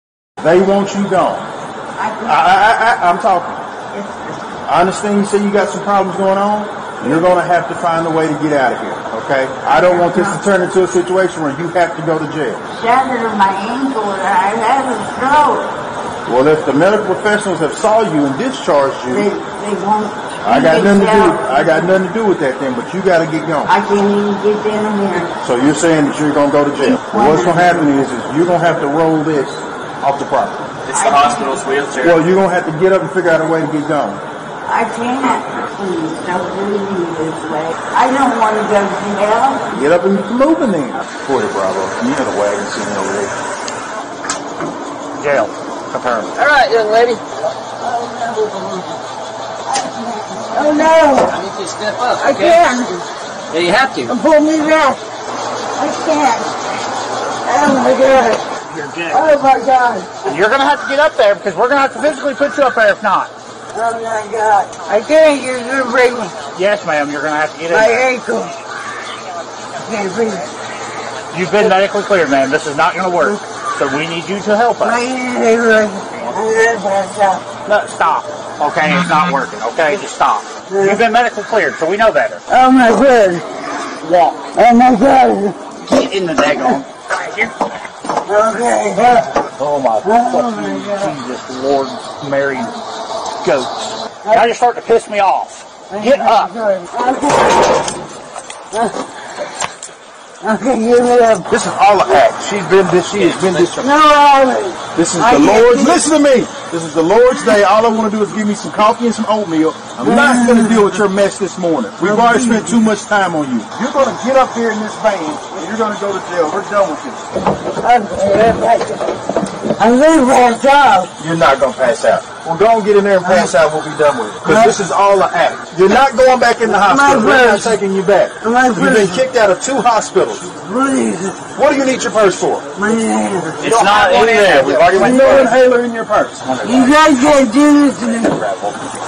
Lisa Edwards, 60, dispensada de dois hospitais sem atendimento nos EUA Lisa morreu na viatura.
Lisa Edwards, 60 anos, dispensada de dois hospitais sem atendimento nos EUA após sofrer derrame e quebrar tornozelo. Ela não tinha plano de saúde. Um dos hospitais chamou a polícia para prendê-la pq ela insistiu em ser atendida. Lisa morreu na viatura.